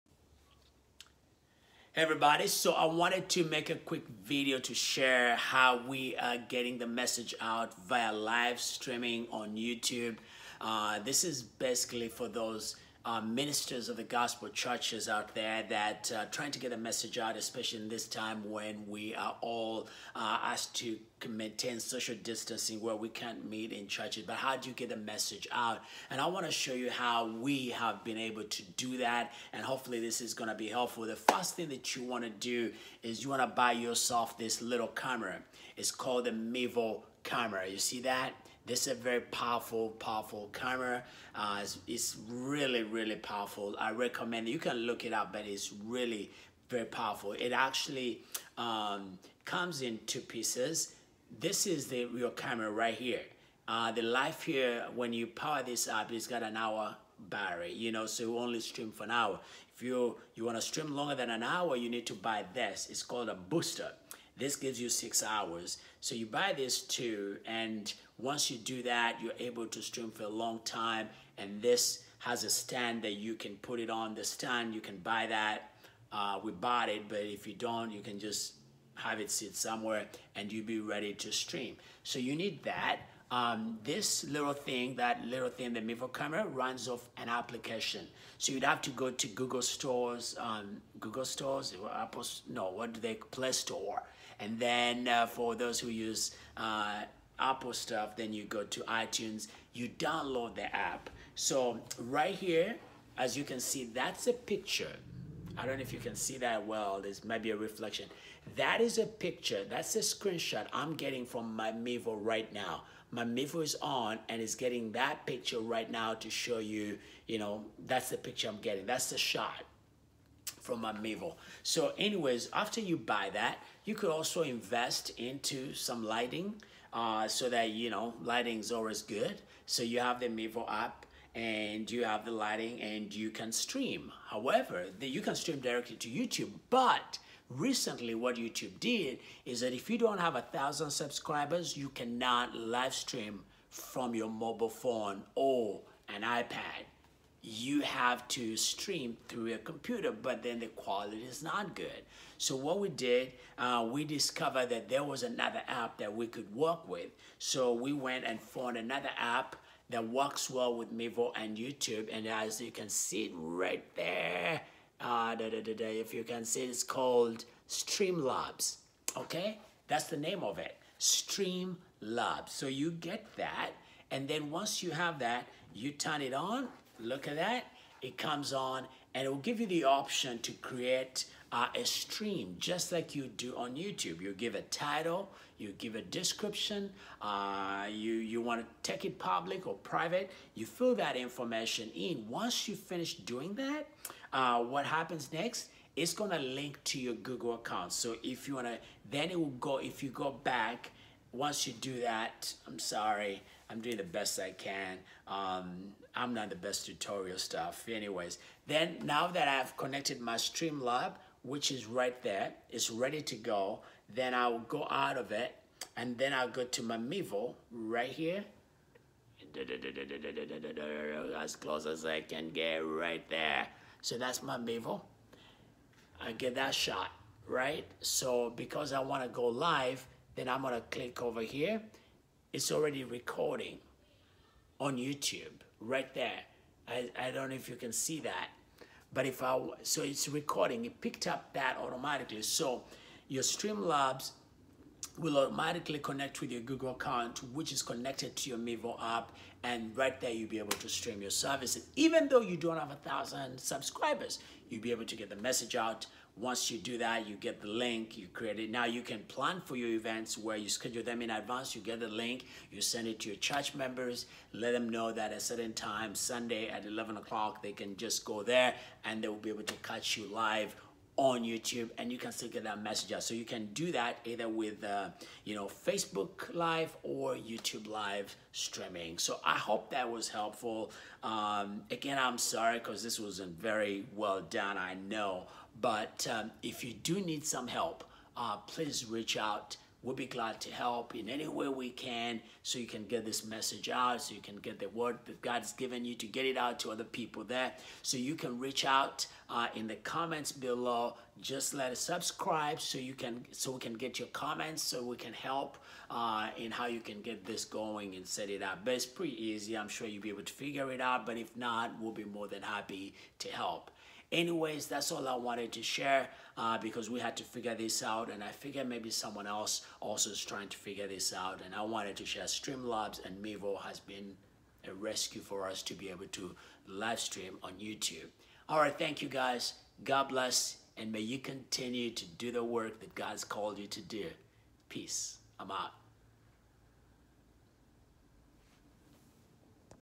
How to Livestream for Churches under 1000 Subscribers Using Mevo Camera.
Here is how you can livestream on YouTube using a Mevo camera in these days of social distancing.

1. Buy Mevo Camera
3. Create a YouTube account if you don’t have one.
4. Download the Streamlabs app
5. To Livestream on YouTube
   - connect your livestream to your YouTube account and go live.